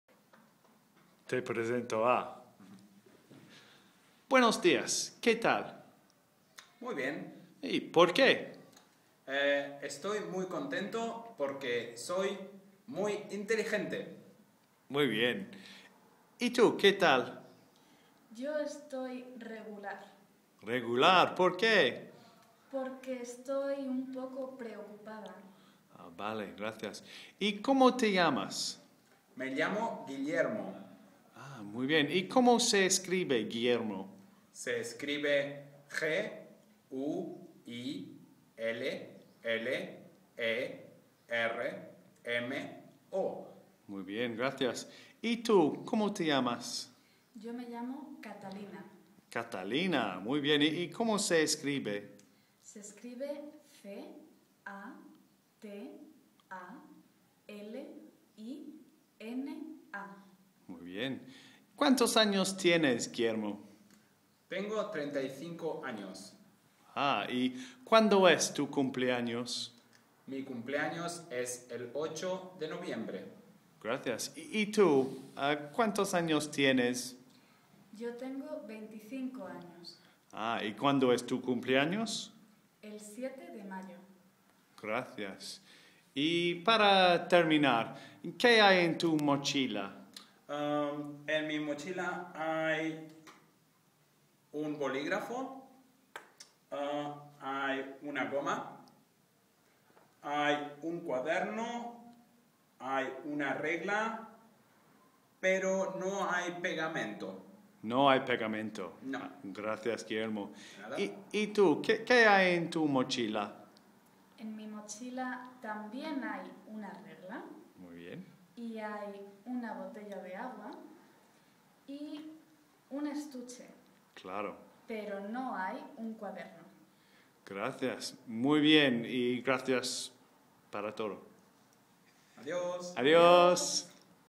64250MFL Amigos 1 Te Presento Conversation
A listening based on giving and receiving personal details.